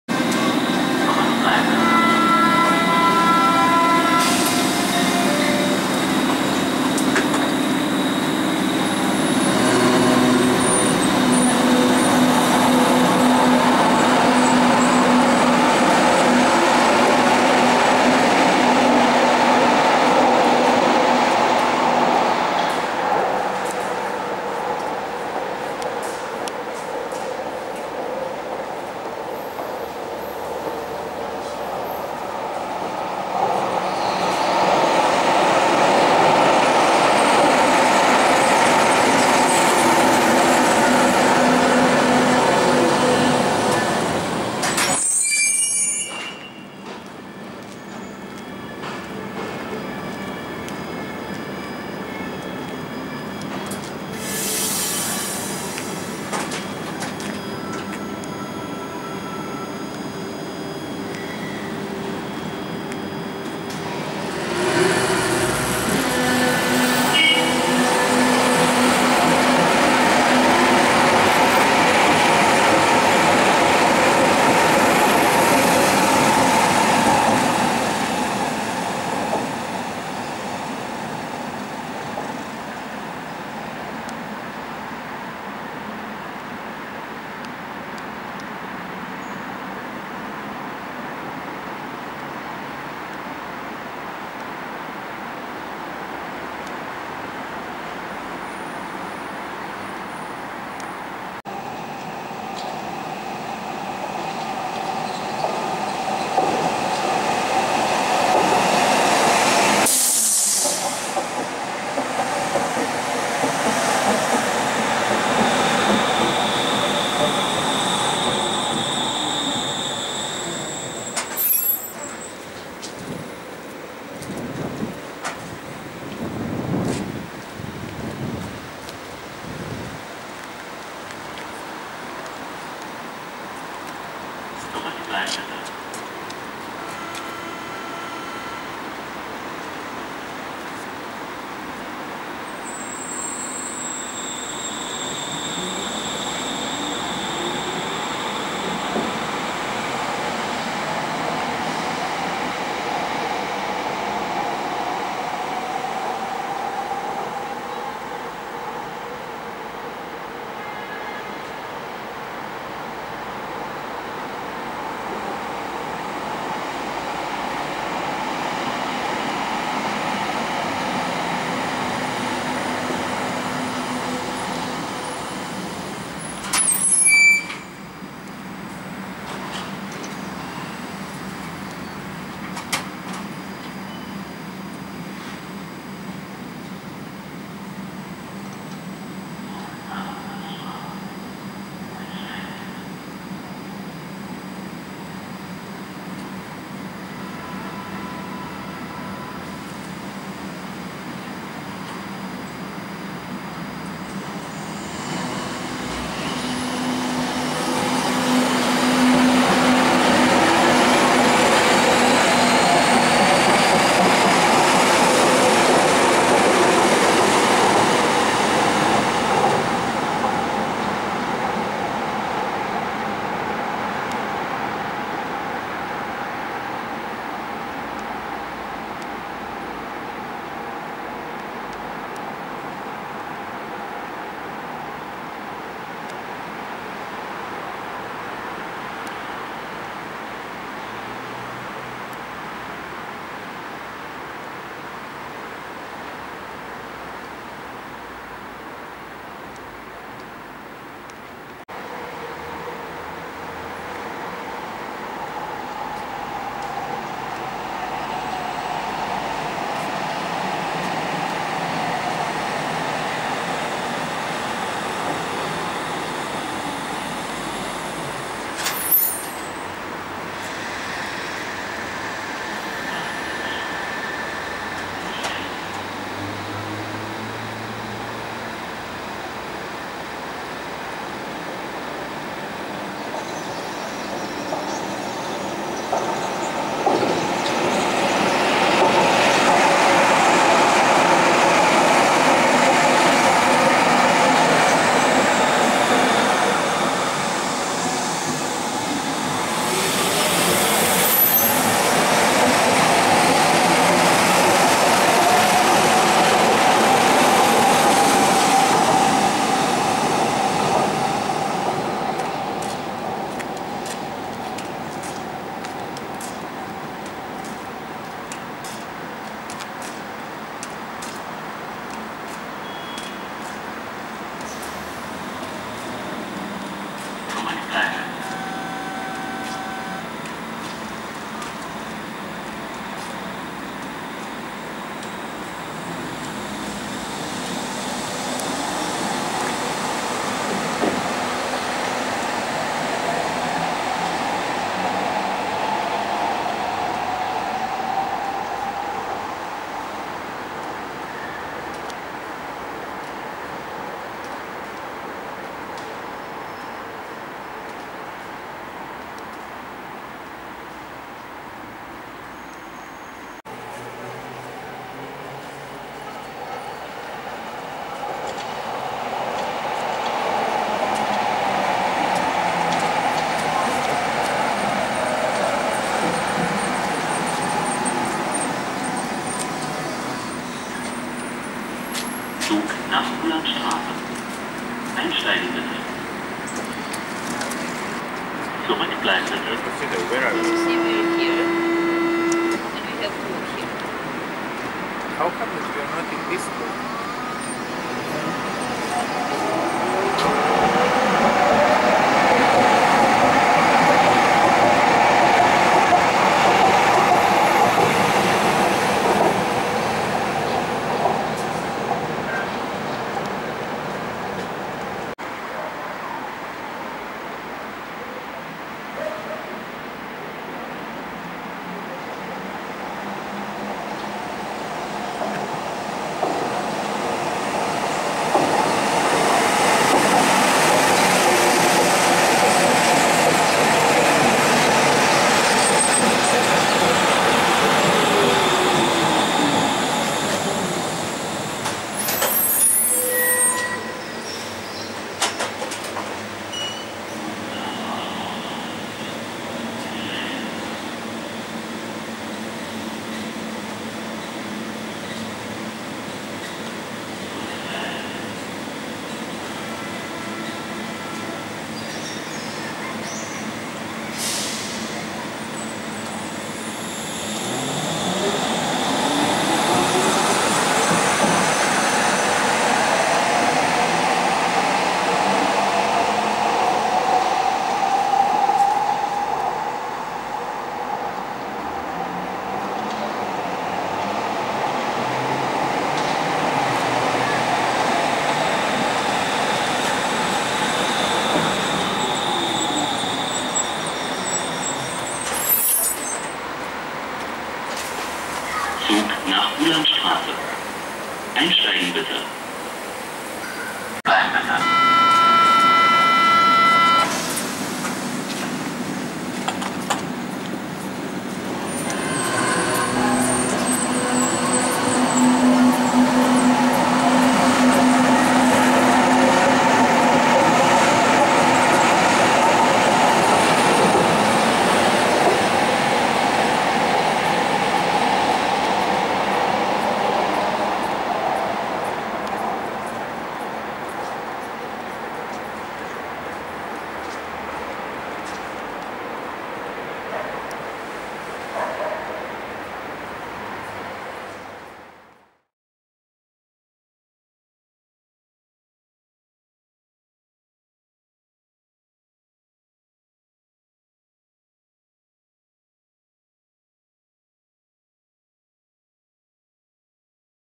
U-Bahn Berlin - U-Bahnhof Prinzenstr. U1 [HD1080p]
Ein kleiner Abstecher zum U-Bahnhof Prinzenstr. auf der Hochbahn der U1.
Mit Zügen der Baureihe A3L92, A3L82 und A3E.

Viel Spaß wie immer :)